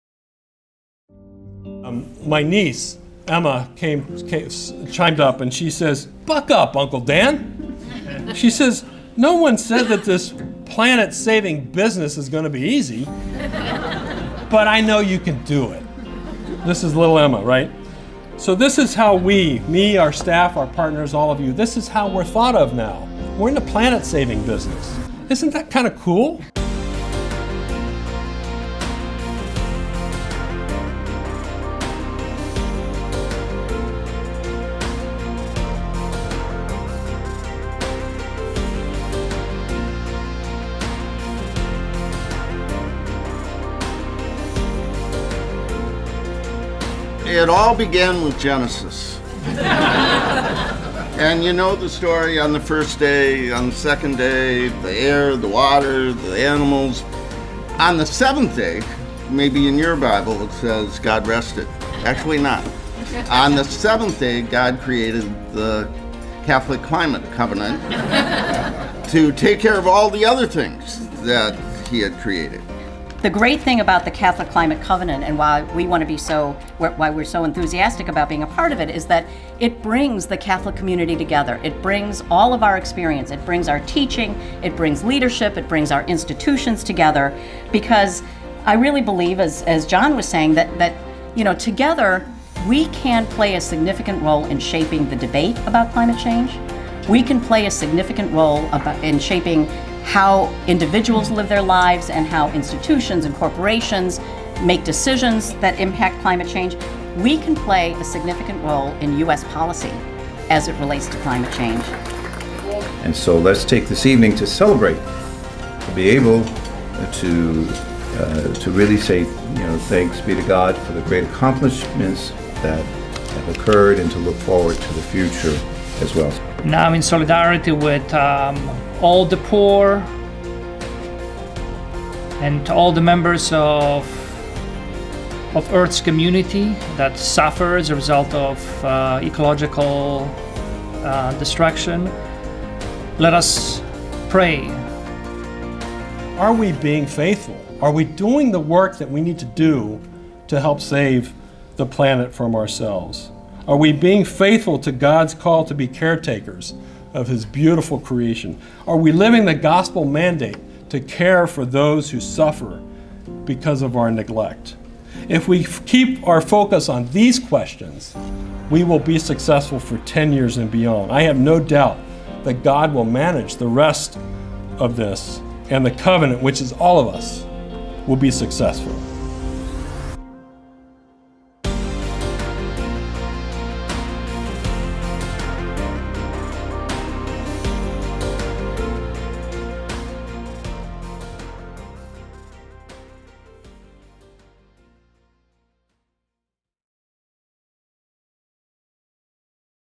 Why Catholic Climate Covenant?
On June 23, 2016 Catholic Climate Covenant celebrated its 10 year anniversary and Laudato Si's 1 year anniversary.